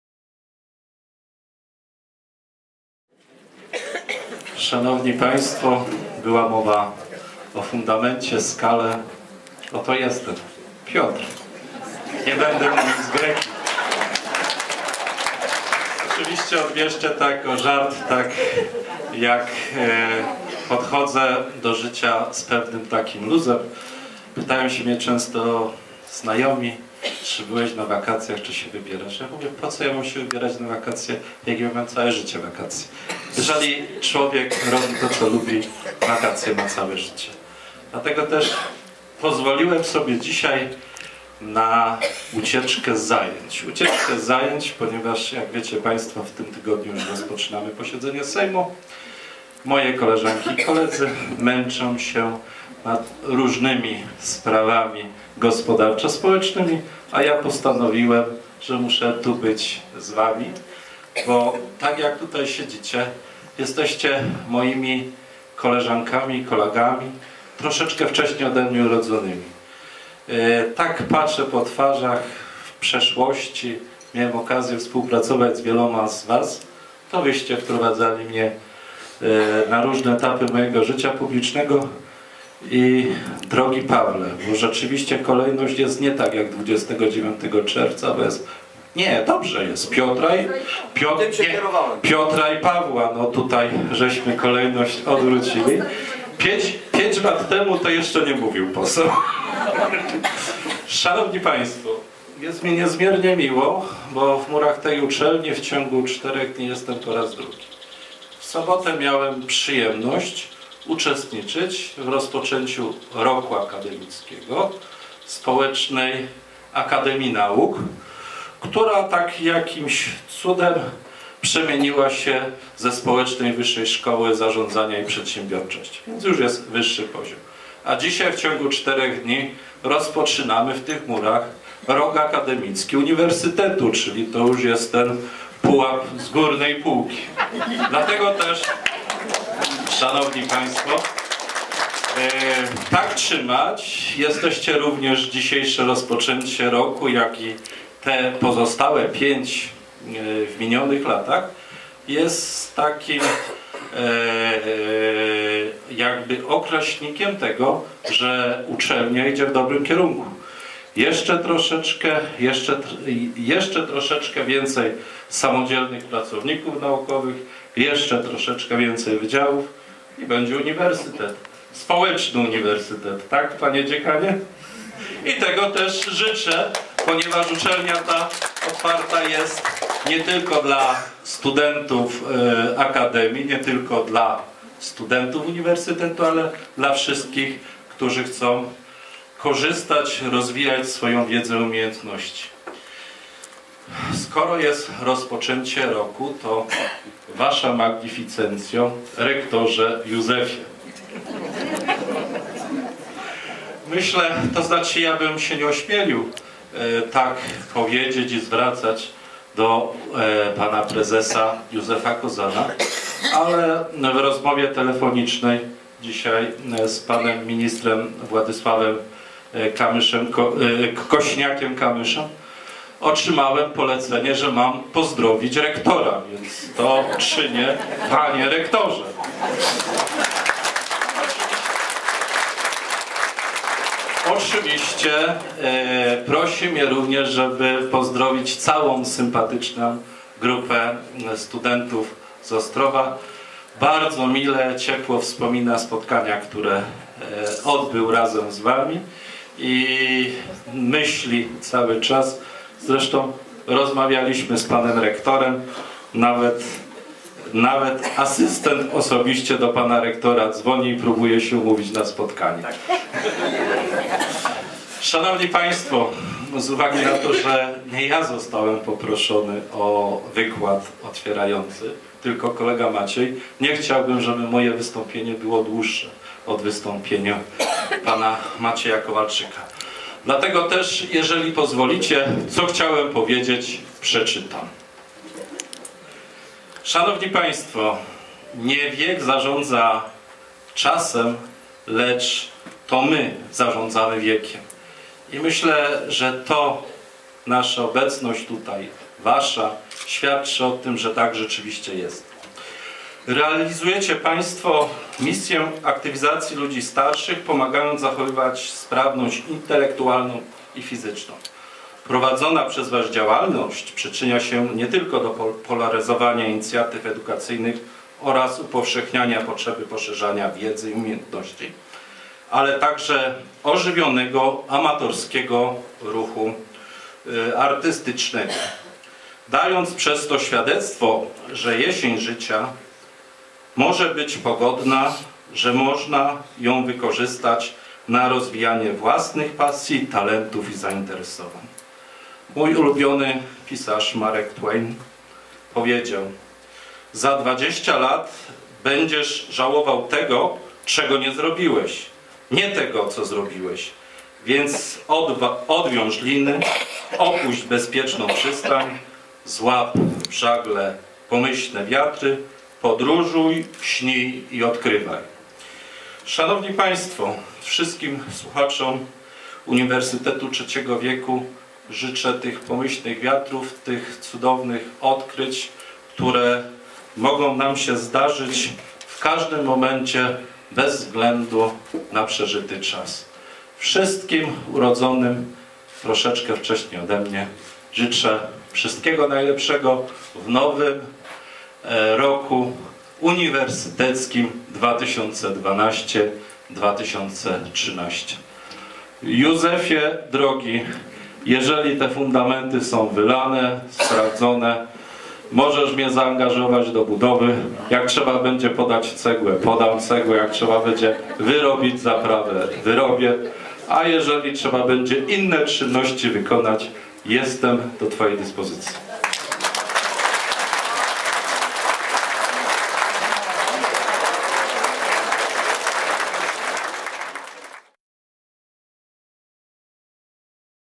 Podam cegłę - pomogę - Piotr Walkowski
Ostrów Wlkp.,Ostrowski Uniwersytet Trzeciego Wieku. Podczas inauguracji roku akademickiego 2012/2013 wystąpił poseł na Sejm Rzeczpospolitej z ramienia PSL Piotr Walkowski z zapewnieniem , że prezes OUTW i sama uczelnia może na niego zawsze liczyć: jako Piotr - Opoka pomogę na ile będę mógł: podam cegłę, urobię wapno.